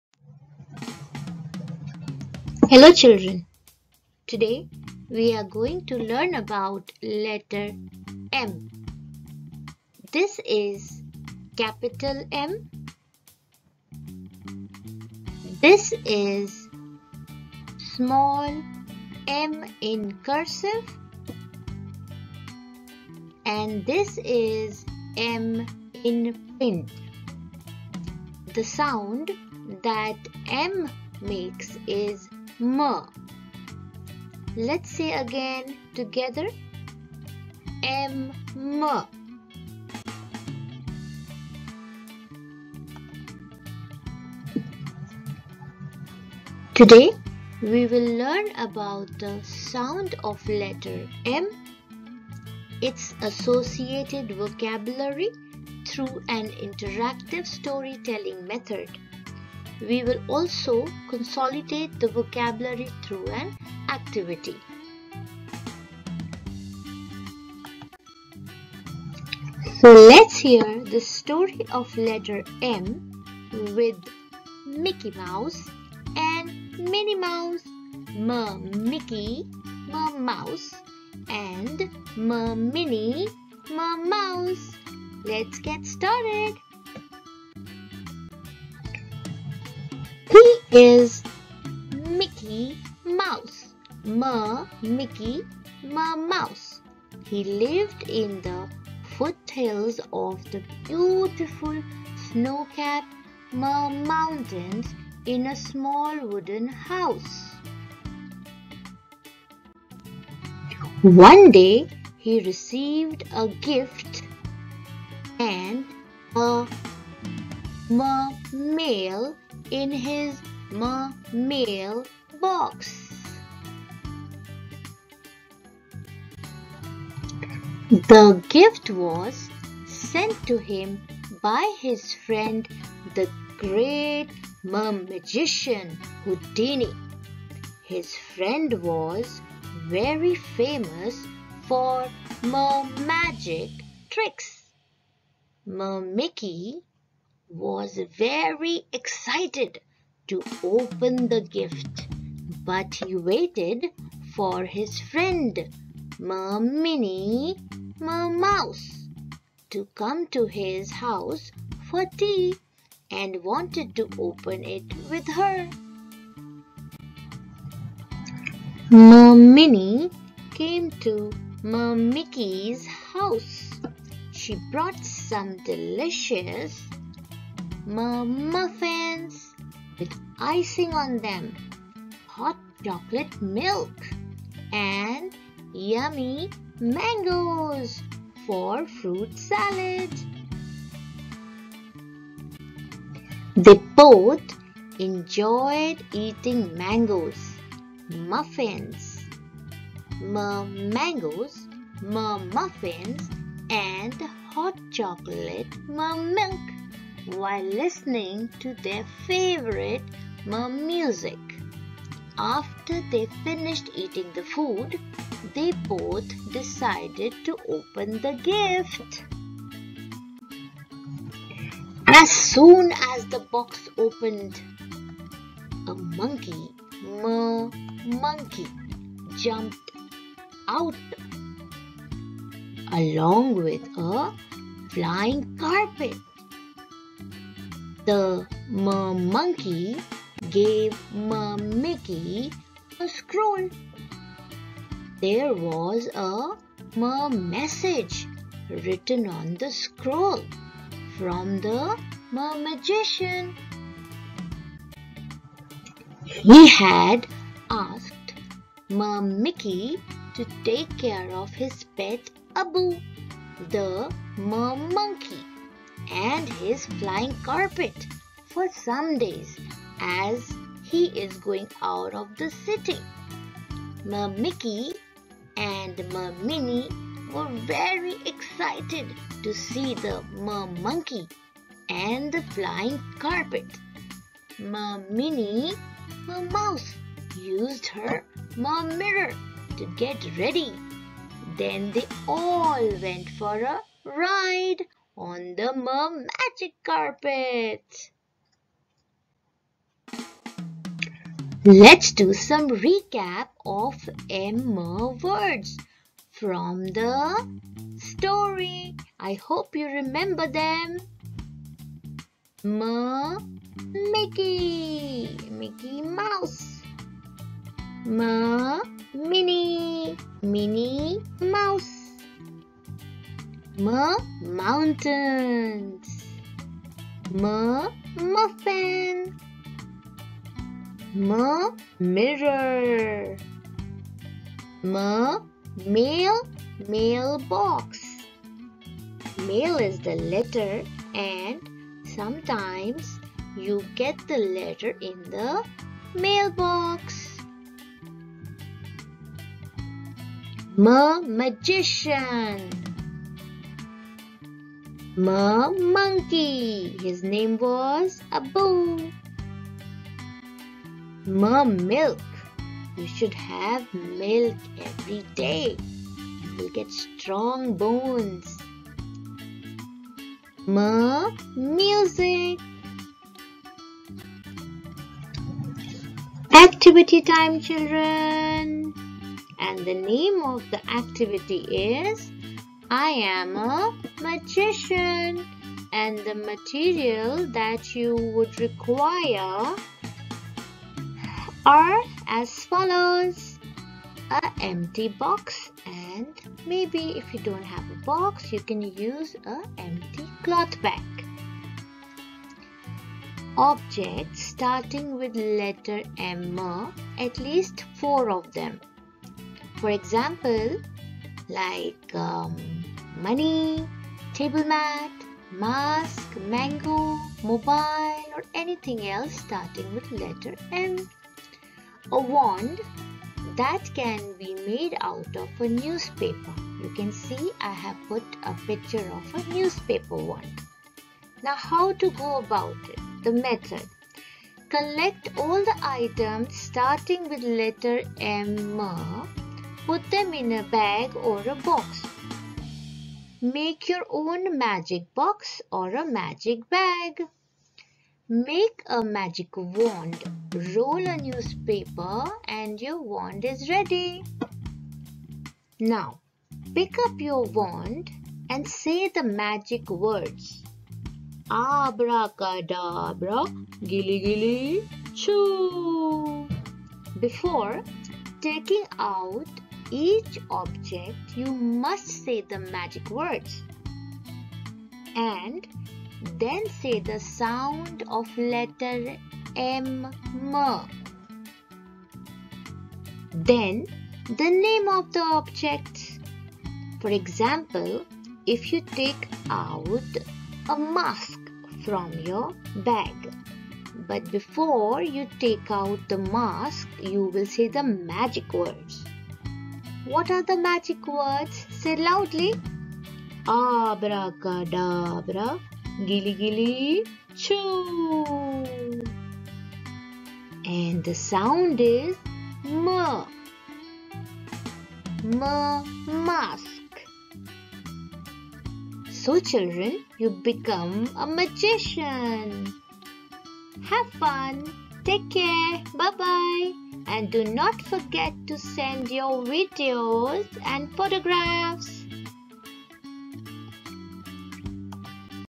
Letter'M m' with Amardeep
Enjoying learning words associated with the letter 'M m'. Watch till the end for an amazing activity.